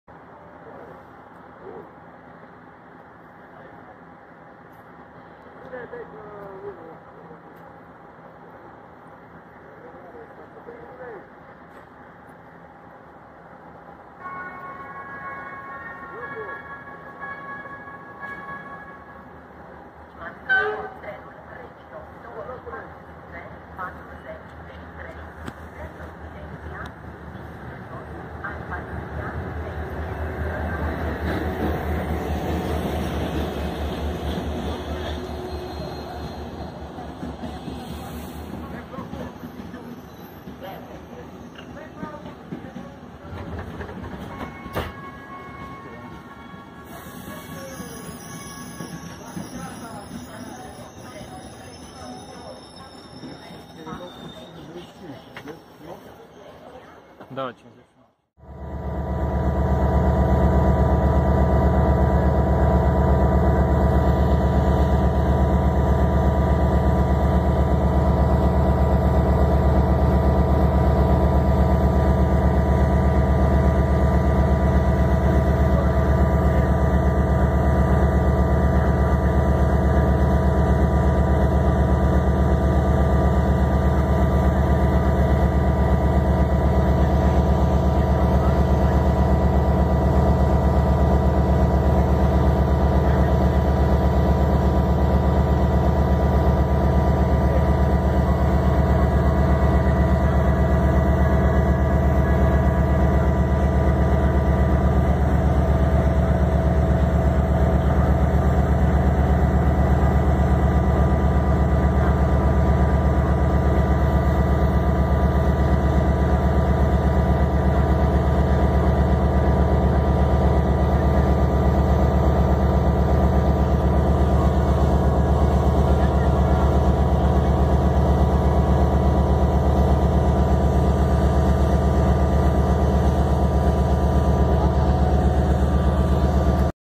Activitate feroviară în gara Sibiu
*Sosire IR1623 (București Nord - Sibiu) cu EGM943 alias "Irina" la tracțiune.
*Cuplare EGM1190 la garnitura trenului IR1624 (Sibiu - București Nord)
*Filmare realizată din R2070 (Sibiu - Craiova)